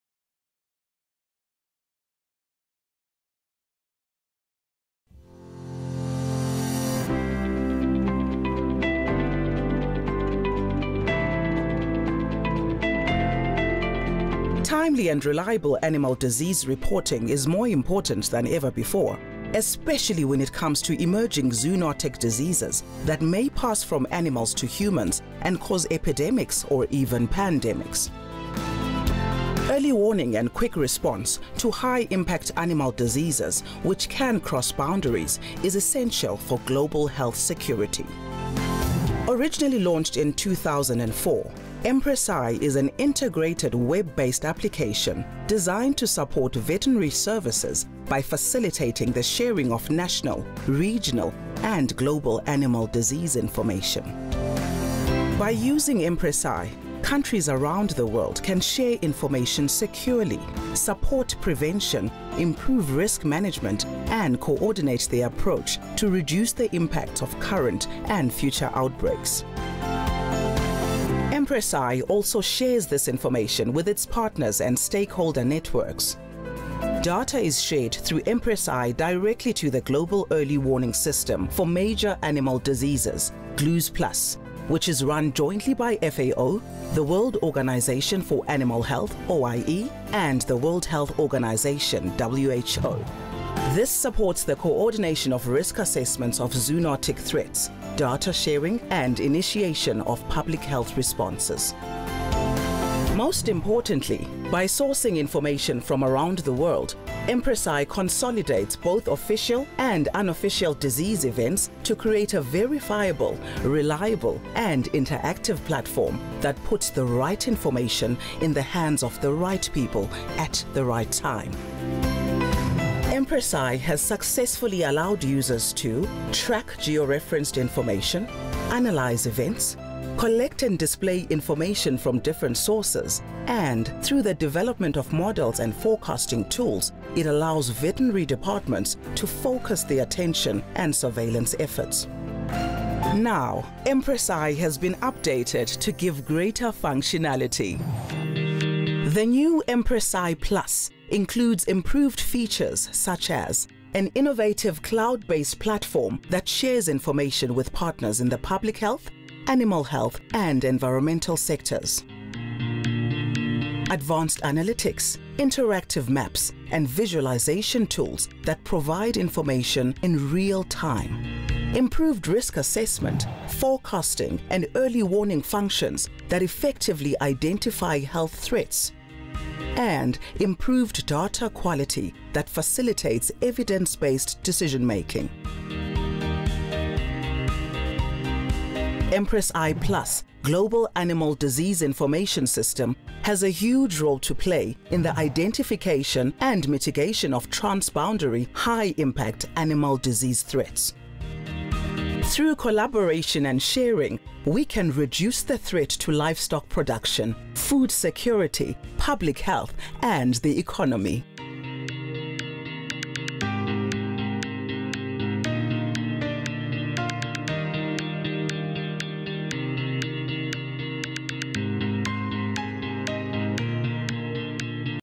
FAO's EMPRES Global Animal Disease Information System (EMPRES-i+)
EMPRES-i+ is FAO’s new global animal disease information system that aims to improve intelligence, forecasting and early warning, enabling countries to monitor disease spread and risk of new outbreaks. This video gives an overview of the EMPRES-i+ and explains new features that users will be able to benefit from.